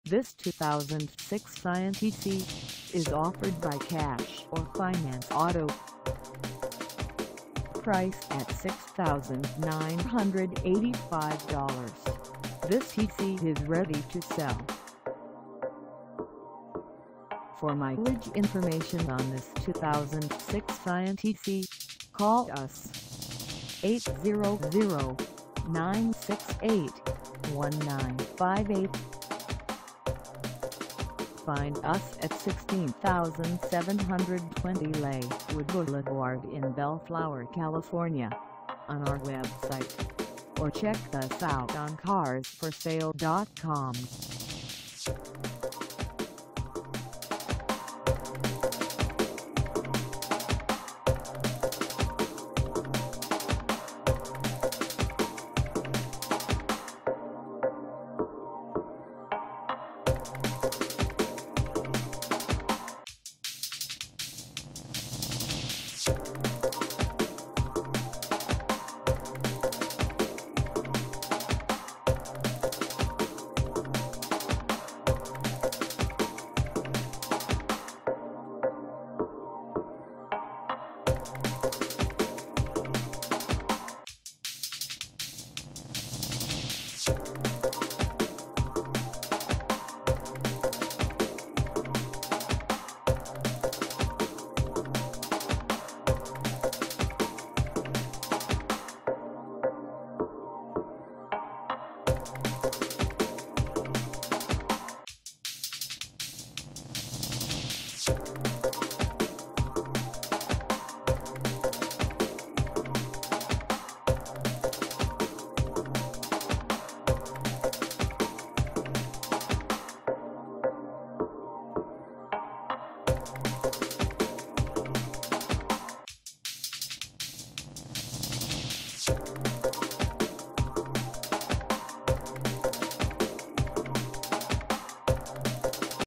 2006 Scion tC Base 2dr Hatchback (2.4L I4 5M) for sale in BE
This 2006 Scion tC Base 2dr Hatchback (2.4L I4 5M) is for sale in BELLFLOWER, CA 90706 at Cash or Finance Auto.
Engine: I4 2.4L I4
Transmission: Manual 5-Speed
Color: Silver
Mileage: 0
Contact Cash or Finance Auto 16720 Lakewood Blvd BELLFLOWER, CA 90706 to test drive this 2006 Scion tC Base 2dr Hatchback (2.4L I4 5M) today.